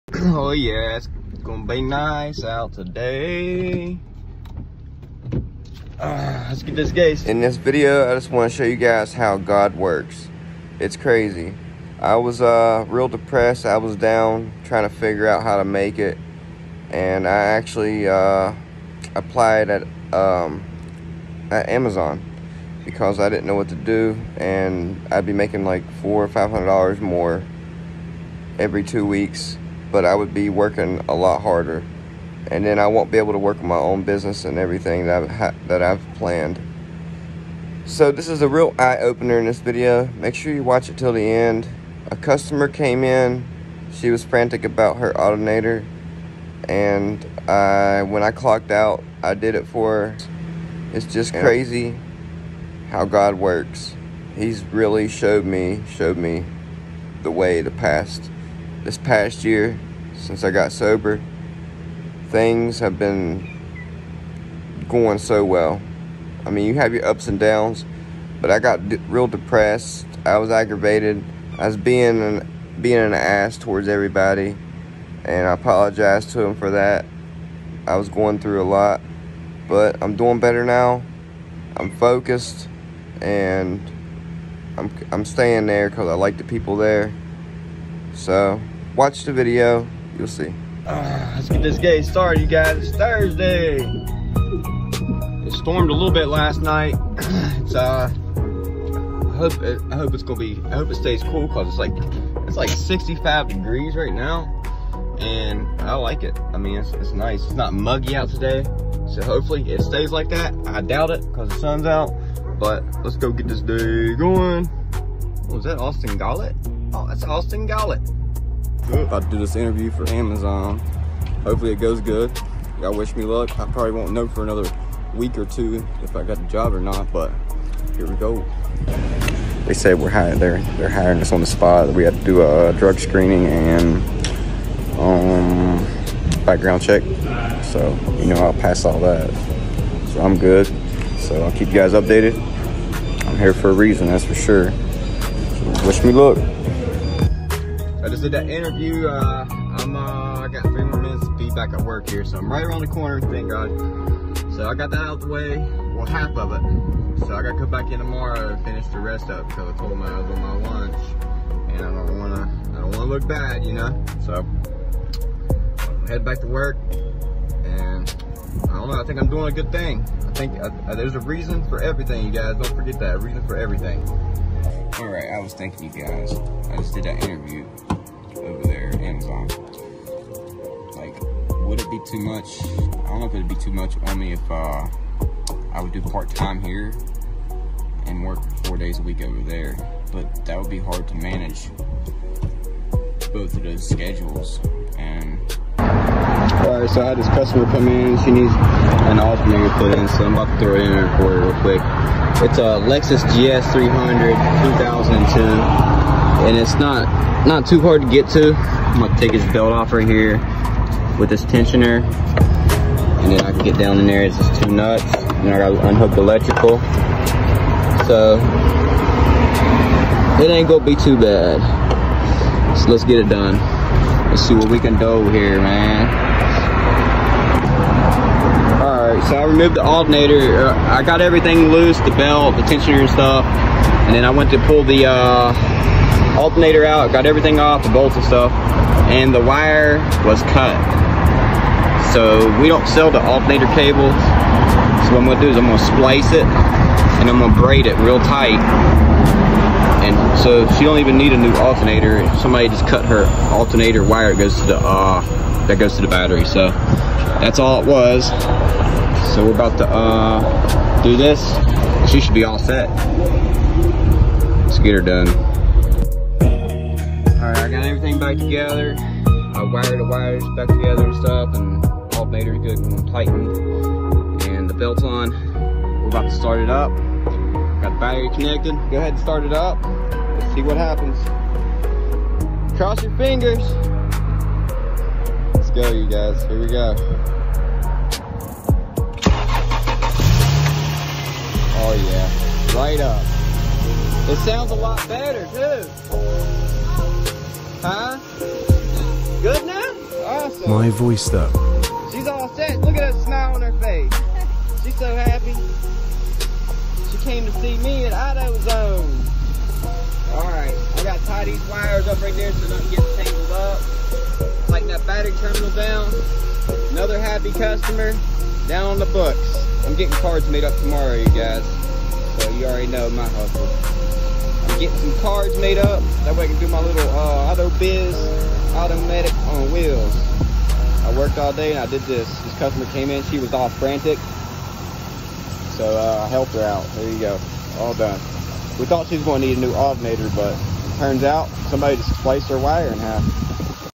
Everyday is a blessing.
I was ready to quit my job and move on to Amazon. Give up everything that I’ve already built. I was going through a lot quitting alcohol marijuana and tobacco at the same time so my anxiety was through the roof and plus not being able to stretch my money from check to check this customer came in which was my real first official Mechanic job. It’s crazy how God works right when I’m ready to give up something positive happens which I always try to stay positive but everybody knew something was going on.